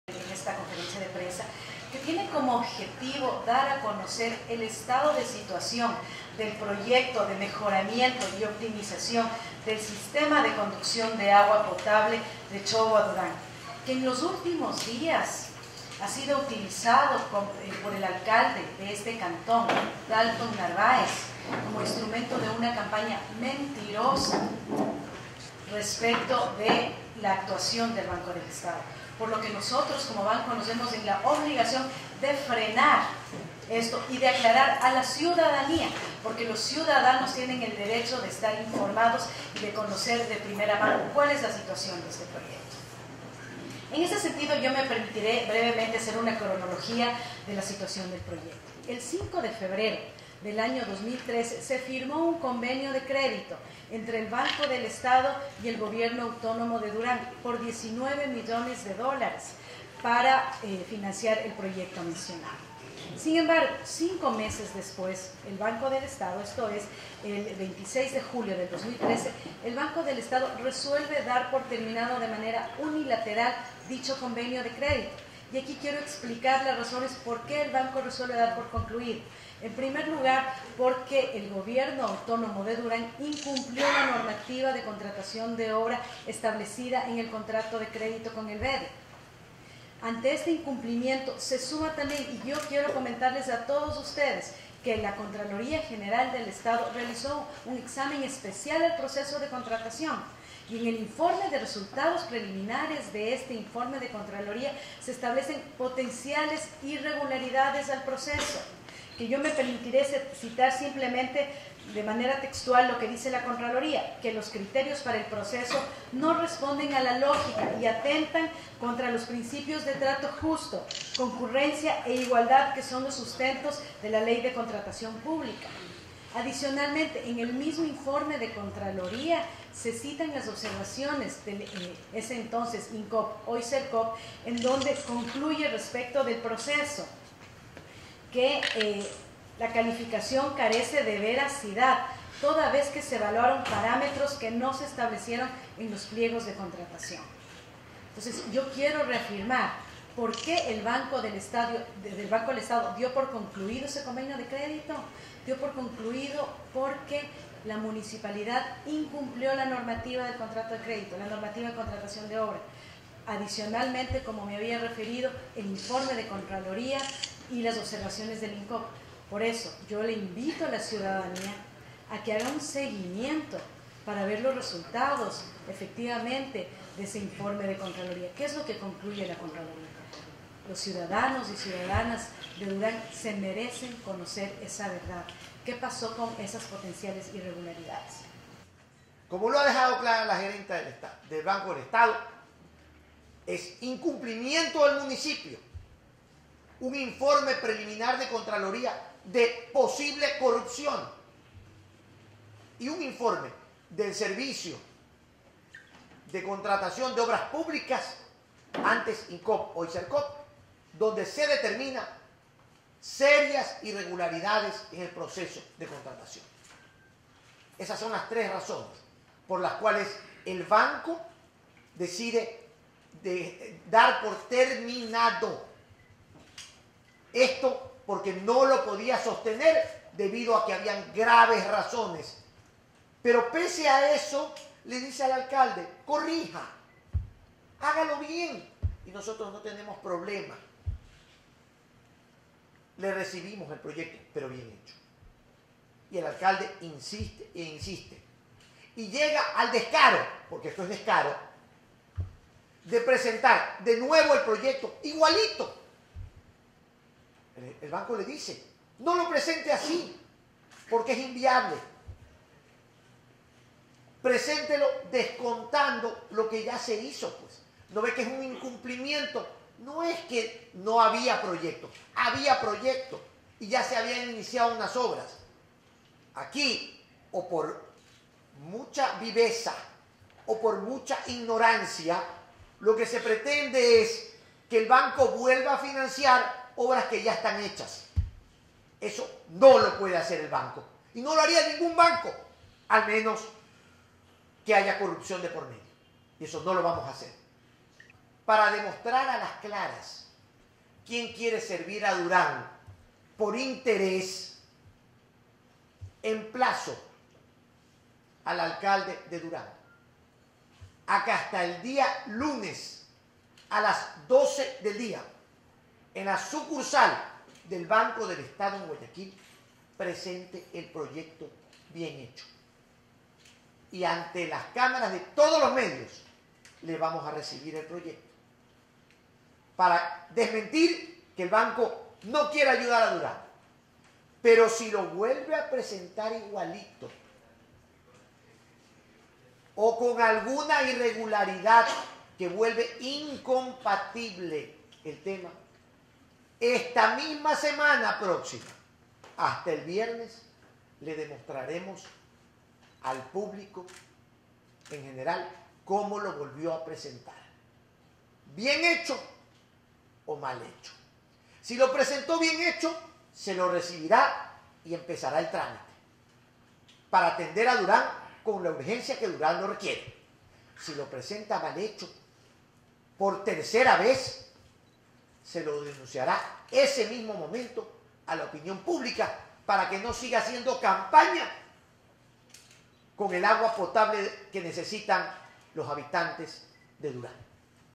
Gobernador emplaza a Alcalde de Durán a presentar nuevo proyecto de conducción de agua
El alcalde de Durán, Dalton Narváez, fue emplazado por el Gobernador del Guayas, Rolando Panchana Farra, a presentar hasta el próximo lunes 3 de febrero una versión actualizada del  proyecto para la conducción de agua Chobo-Durán. El requerimiento se realizó luego de que el burgomaestre alegase que la ejecución del mismo fue cancelada producto de tecnicismos y diferencias políticas.